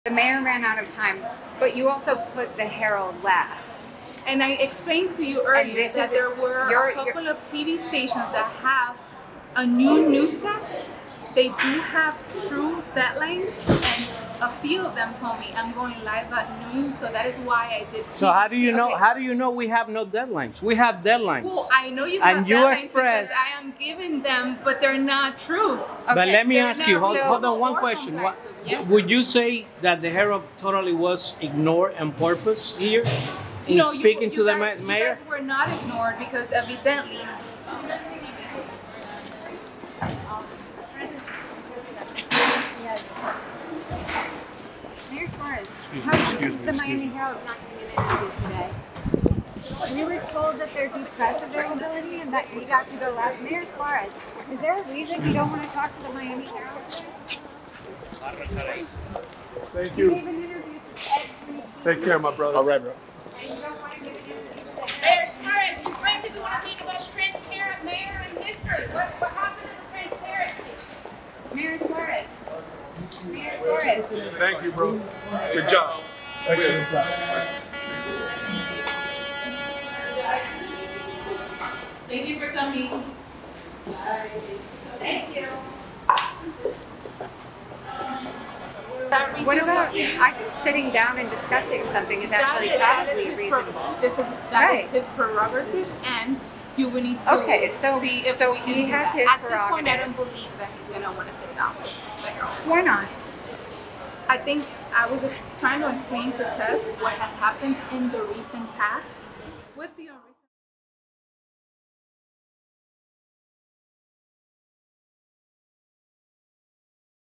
Miami Mayor Suarez avoids questions on side jobs after speech
Miami Mayor Francis Suarez refused to answer Miami Herald questions about his work for Redivider/NZero after his 2024 State of the City address.